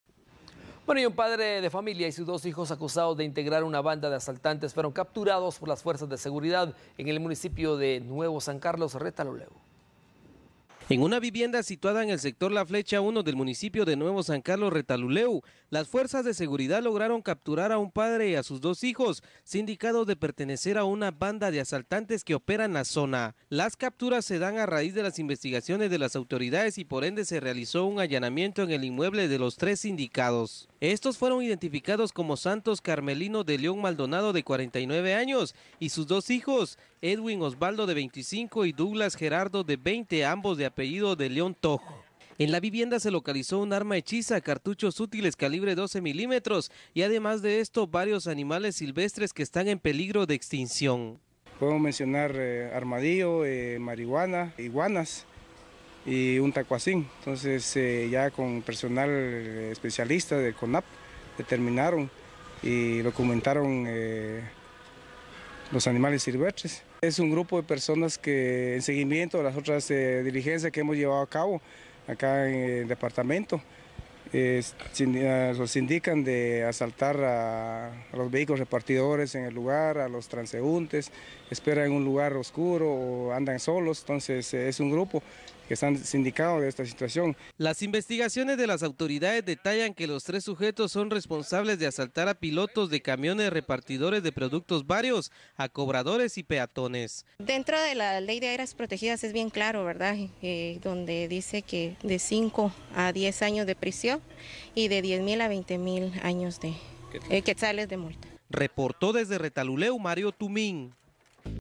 Capturan a padre e hijos acusados de pertenecer a banda de asaltantes
Un padre de familia y sus dos hijos son acusados de integrar una banda de asaltantes. Ellos fueron capturados por fuerzas de seguridad en el municipio de Nuevo San Carlos, Retalhuleu. Se les incautó armas y animales silvestres.